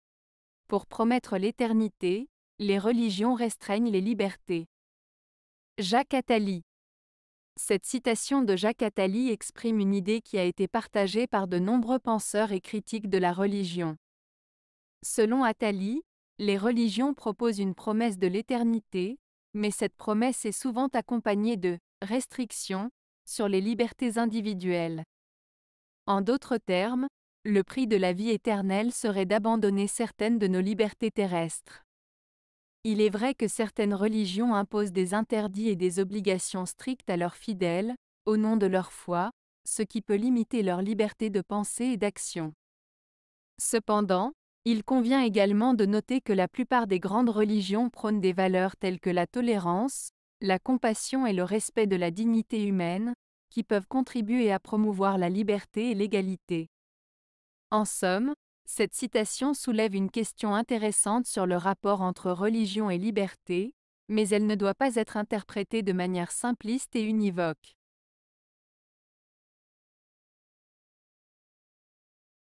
Pour promettre l'Eternité, les religions restreignent les Libertés. - Jacques Attali
Cette citation de Jacques Attali exprime une idée qui a été partagée par de nombreux penseurs et critiques de la religion. Selon Attali, les religions proposent une promesse de l'éternité, mais cette promesse est souvent accompagnée de "restrictions" sur les libertés individuelles. En d'autres termes, le prix de la vie éternelle serait d'abandonner certaines de nos libertés terrestres.
Il est vrai que certaines religions imposent des interdits et des obligations strictes à leurs fidèles, au nom de leur foi, ce qui peut limiter leur liberté de pensée et d'action. Cependant, il convient également de noter que la plupart des grandes religions prônent des valeurs telles que la tolérance, la compassion et le respect de la dignité humaine, qui peuvent contribuer à promouvoir la liberté et l'égalité.
En somme, cette citation soulève une question intéressante sur le rapport entre religion et liberté, mais elle ne doit pas être interprétée de manière simpliste et univoque.